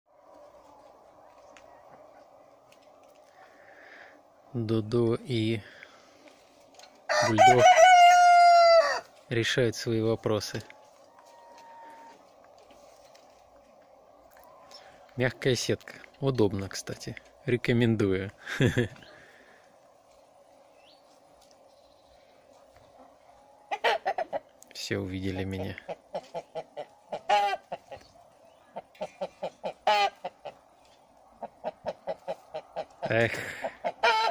Тестостерон зашкаливает)
По правилам Ютуб, петухи не имеют права бится в открытом поединке. Теперь придумали выход– через рыбатскую сеть.